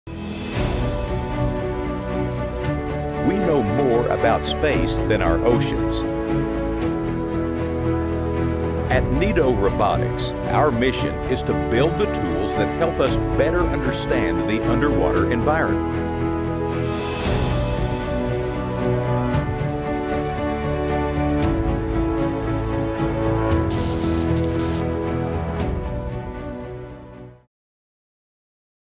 Sibiu Nano Underwater Drone - Teaser
✅ Carry out inspections down to 100m below surface
✅ Collect Scientific Data
✅ Teach Robotics and STEM
✅ Record and stream live video from undersea
✅ Explore the oceans